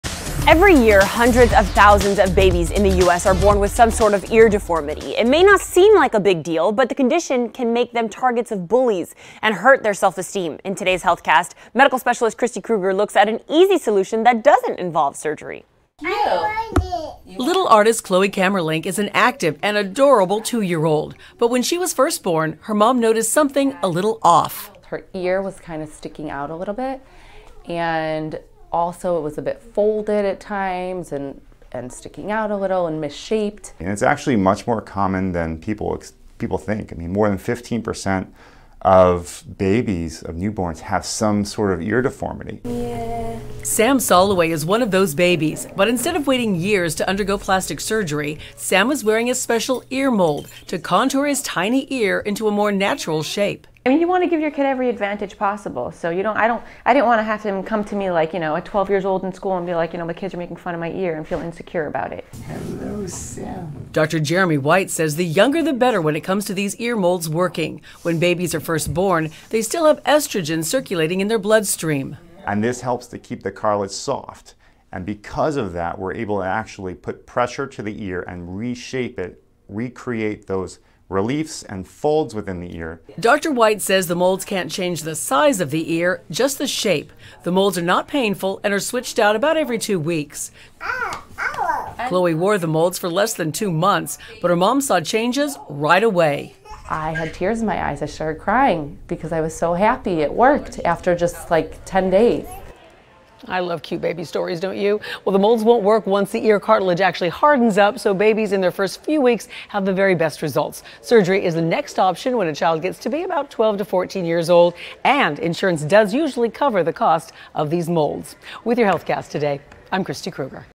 Ear molds help contour babies' ears into more natural shape
About 15 percent of babies are born with some sort of ear deformity, and ear molds are a noninvasive way to contour their tiny ears into a more natural shape.